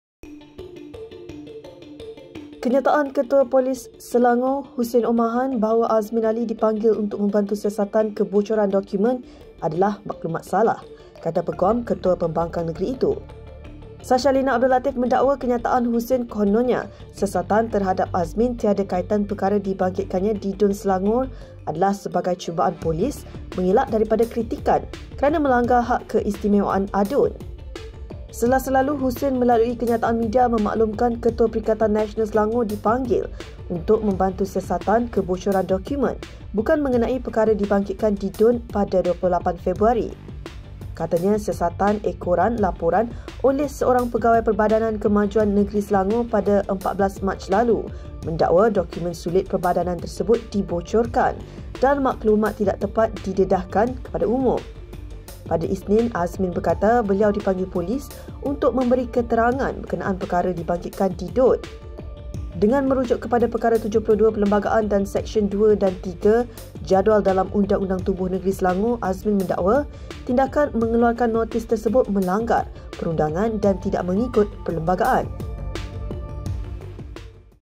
Peguam Azmin dakwa ketua polis Selangor beri maklumat salah
Kenyataan Ketua Polis Selangor Hussein Omar Khan bahawa Azmin Ali dipanggil untuk membantu siasatan kebocoran dokumen adalah maklumat salah, kata peguam ketua pembangkang negeri itu.